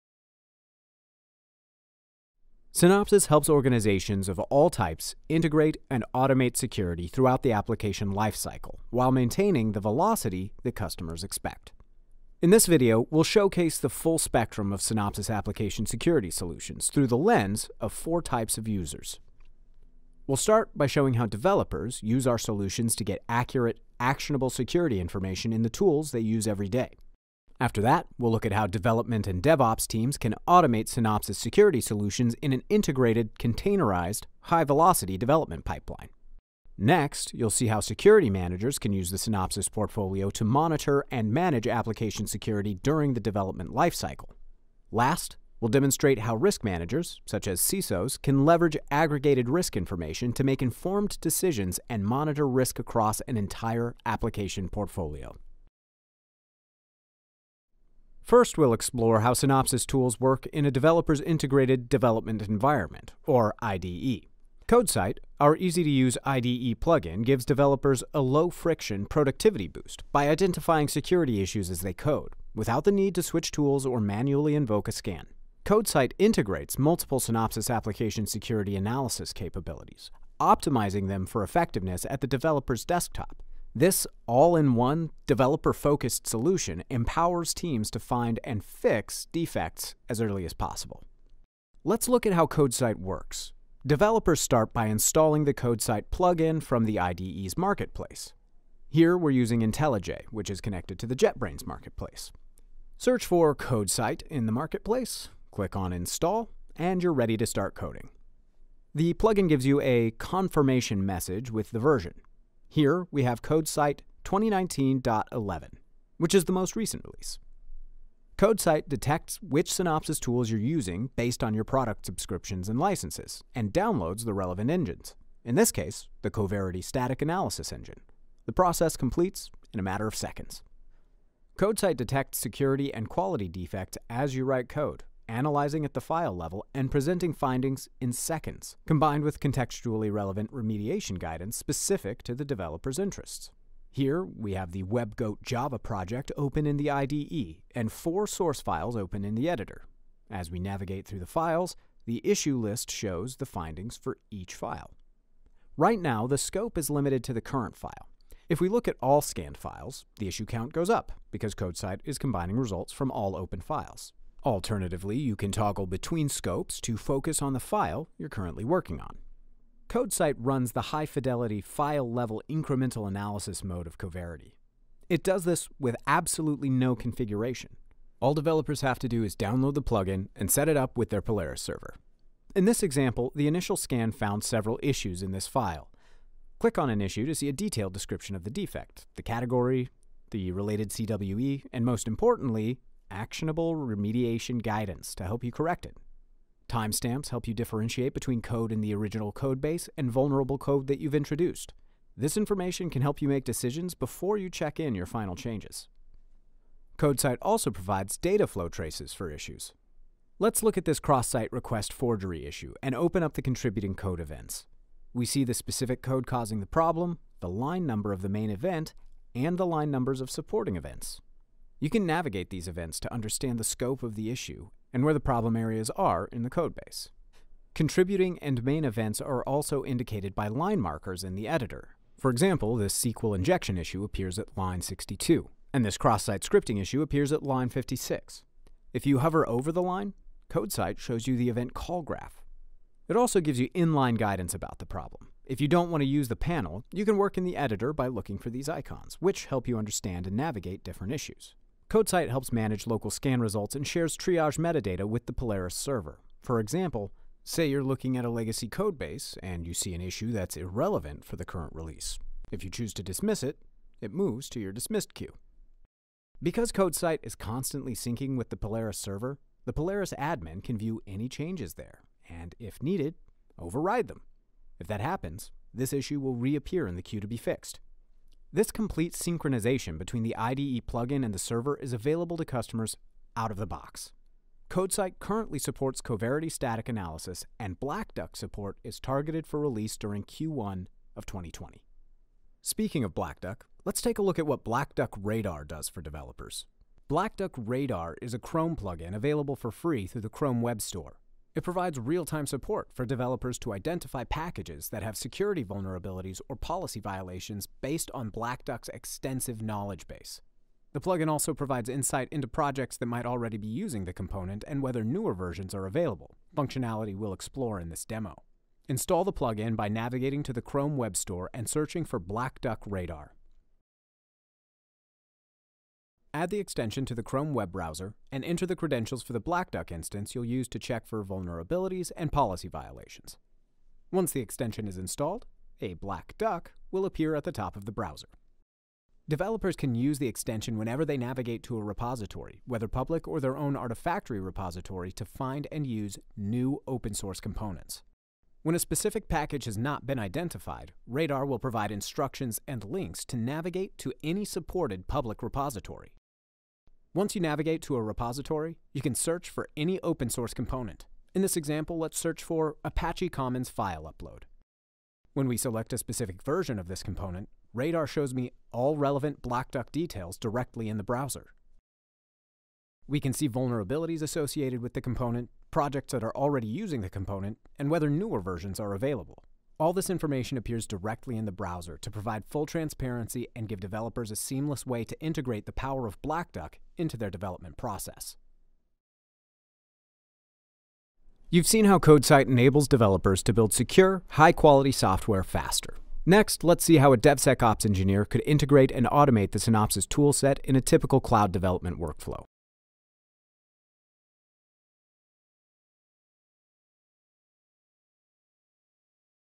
Application Security Solutions for Developers - Full Spectrum 2020 | Black Duck
A highlight video highlighting and demoing how developers can take advantage of some of the Black Duck application security tool offerings.